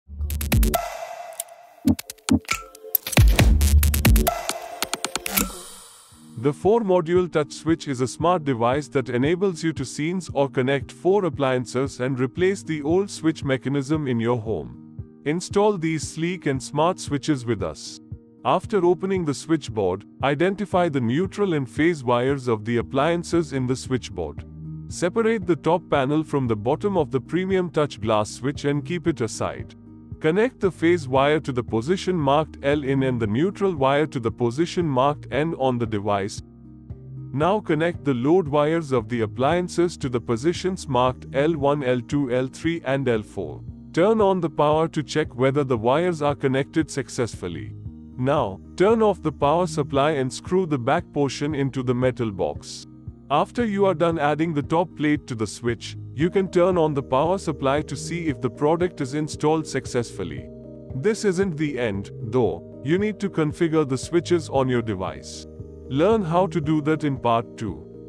Elleys’ Premium Touch Installation guide
If you are automating your home, go that extra mile and make it look luxurious as well, by opting for the PREMIUM TOUCH GLASS PLATE SWITCHES.

These switches allow you to pre-set scenes and allot them to the touch switch, monitor devices, and operate from anywhere through our App, voice assistants (Alexa, Ok Google, etc.), and touch panels.

This is how you install the Premium Touch Glass Plate Switches:

After opening the switchboard, identify the neutral and phase wires of the appliances in the switchboard.

Separate the top panel from the bottom of the premium touch glass switch and keep it aside.
Connect the phase wire to the position marked L-in and the neutral wire to the position marked N on the device.

Now connect the load wires of the appliances to the positions marked L1, L2, L3, and L4. Turn on the power to check whether the wires are connected successfully.
Turn off the power supply and screw the back portion into the metal box.
Once you are done with the installation, go ahead and configure the switches and start enjoying the luxury and convenience it brings!